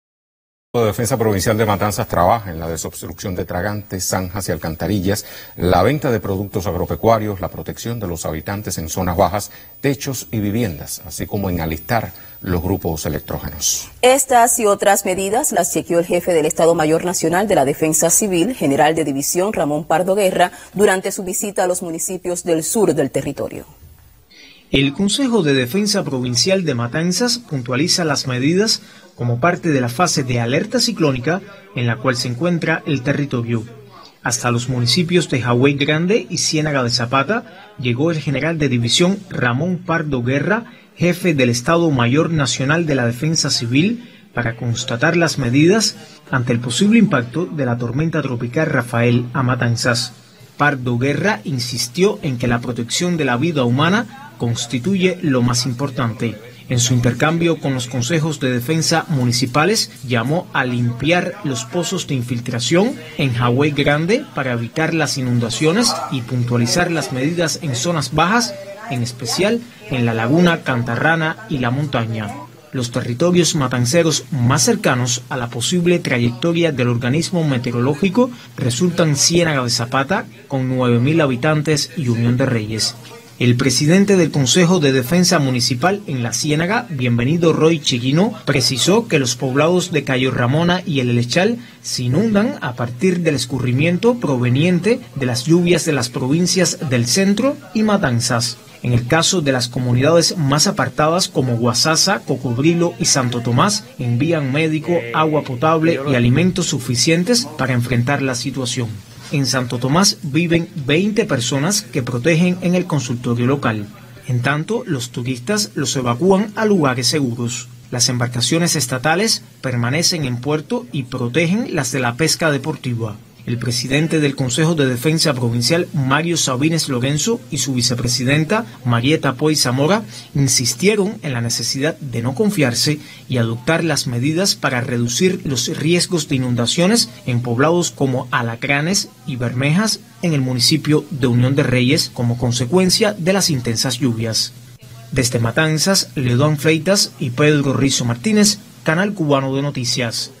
Recorre Ramón Pardo Guerra zona Sur de Matanzas
El Consejo de Defensa Provincial de Matanzas trabaja en la desobstrucción de tragantes, zanjas y alcantarillas; la venta de productos agropecuarios; la protección de los habitantes en zonas bajas, techos y viviendas, así como en alistar los grupos electrógenos. Estas y otras medidas las chequeó el jefe del Estado Mayor Nacional de la DefensaCivil, general de división, Ramón Pardo Guerra, durante su visita a los municipios del sur del territorio de Cuba.